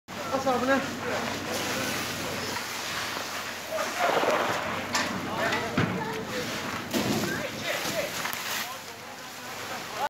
Talvar və qaz sobası yanıb
Alov yanğından mühafizə bölmələri tərəfindən söndürülüb